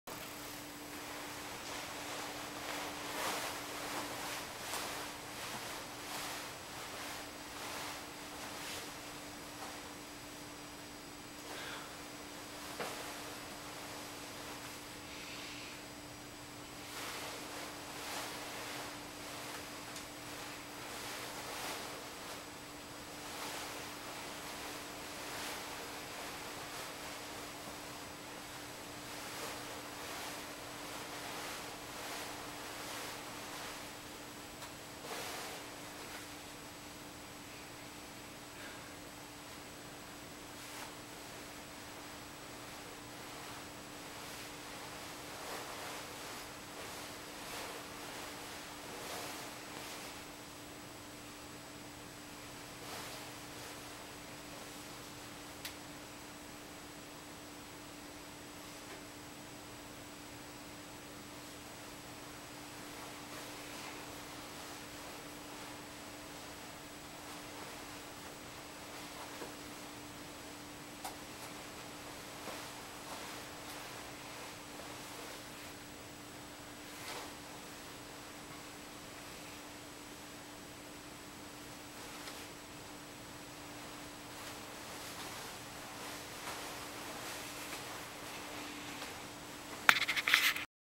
Blue Print One
Nice full skirted blue print with lots of petticoats.  I loved wearing it.